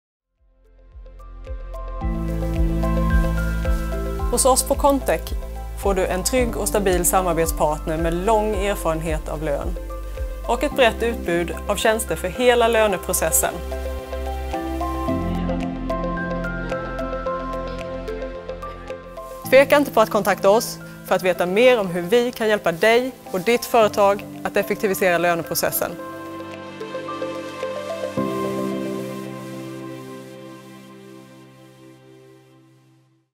Kontek
En liten film vi producerade för Kontek Lön AB hösten 2021, den handlar om Konteks erbjudande och gjordes i två längder varav denna är den korta.
Foto & klippning, Martin Östberg